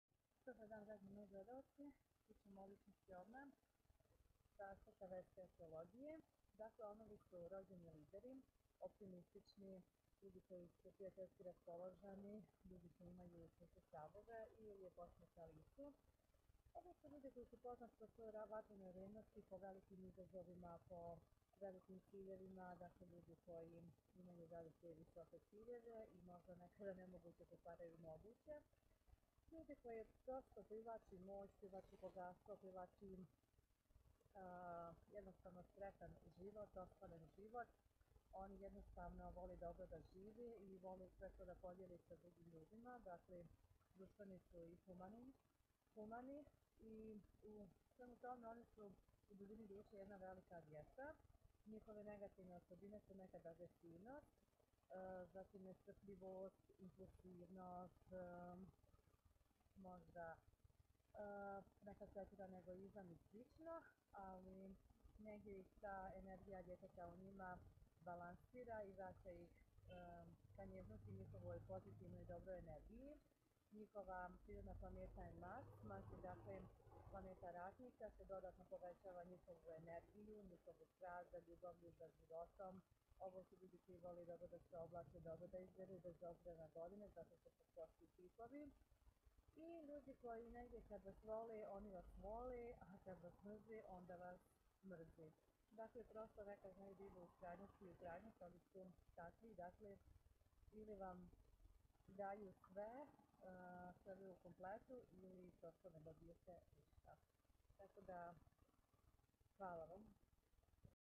"Zivi,voli,leti" Vedska astrologija,Licnost Ovan
Ovnovi su rodjeni lideri,optimistic I,strastveni,vole moc,bogastvo,avanturu,slobodu ,prijateljski su raspolozeni I imaju blag osmjeh na licu.Poznati po svojoj vatrenoj revnosti,odlucni u ostvarivanju svojih ciljeva I velikih zadataka.Oni se dobro oblace ,vole da zadrze dobar izgled bez obzira na godine.Ne vole lijenost,neaktivnost,negativnost.Njihova prirodna planeta Mars ,planeta ratnika povecava njihovu strast I energiju ratnika I pobjednika.Negative osobine nestrpljivost,tvrdoglavost,impulsivnost.
Ovo su ljudi koji kad vole vole a kad mrze onda mrze.Hvala